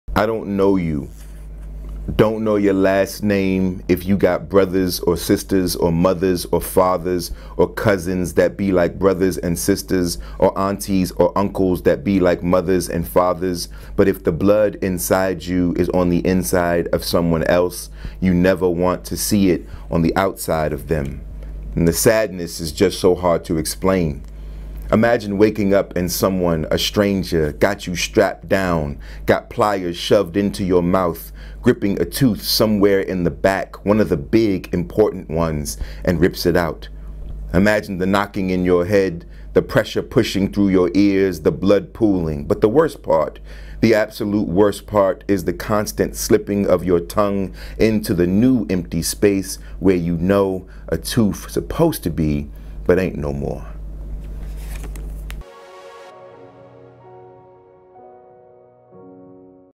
Jason Reynolds reads from Long Way Down
Jason Reynolds reads an extract from his amazing book Long Way Down.

Filmed and edited by Robbie Hunt